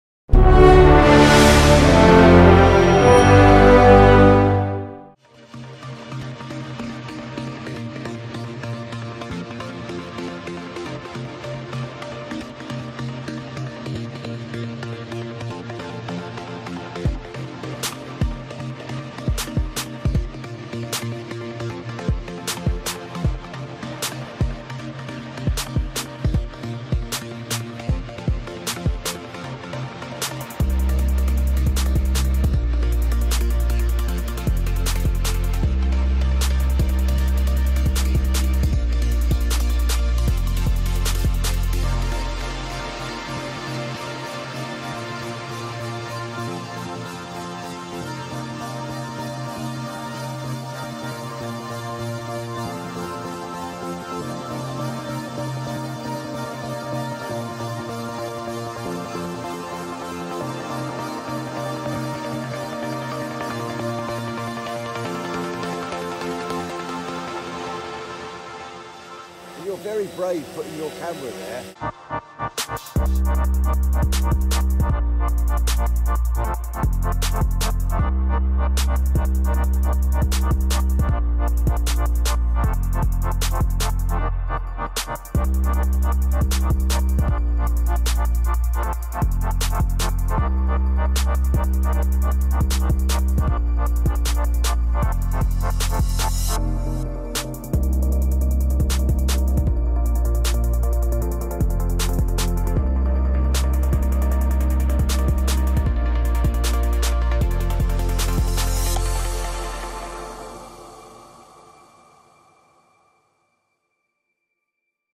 The Quarry
The "Valencia Division" of the Spanish Drone Seekers are out and about again, exploring the Quarry near Vistabella Golf, Orihuela. Had total faith in Terry not crashing into my camera...!